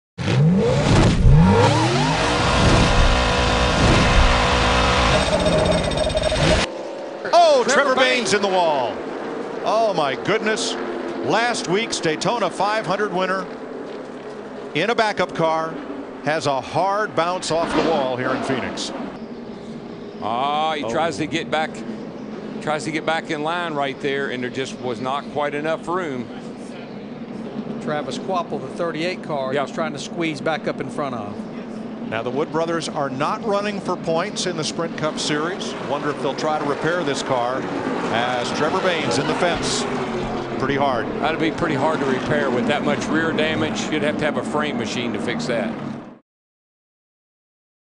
2011 Subway Fresh Fit 500 - Trevor Bayne Crash
Driving in a back-up car, Trevor Bayne turns across Travis Kvapil's nose and turns himself into the wall